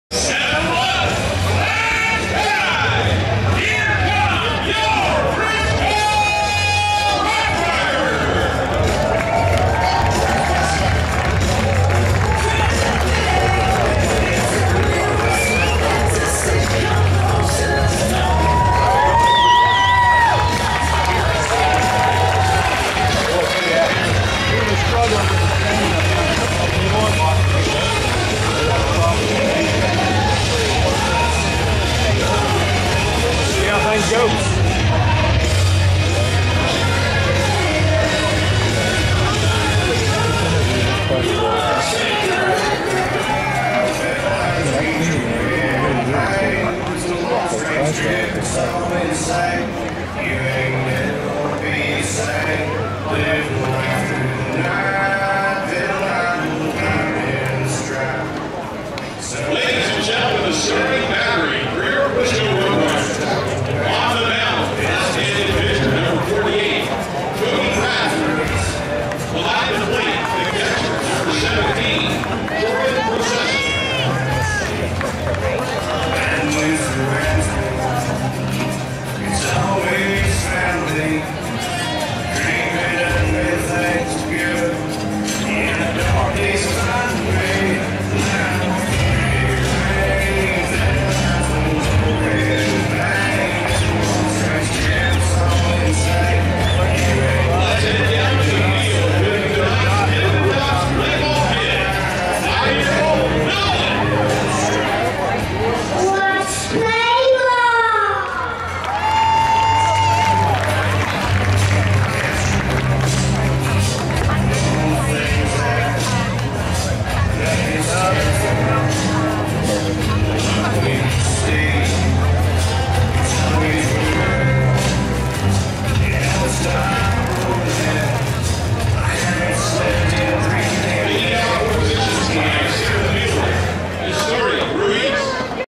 RoughRiders/Missions 9/8/21: Riders Take the Field
The Frisco RoughRiders take the field before the second game of a six-game series against the San Antonio Missions.